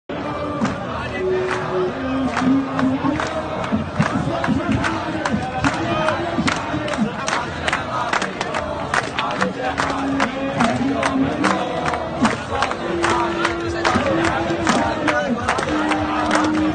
طلبة الجامعات والمعاهد بمحافظة البصرة يضربون عن الدوام ويخرجون بتظاهرات حاشدة
طلبة الجامعات والمعاهد بمحافظة البصرة يضربون عن الدوام ويخرجون بتظاهرات حاشدة تطالب بإسقاط الأحزاب الفاسدة
ـــــــــــــــــــــ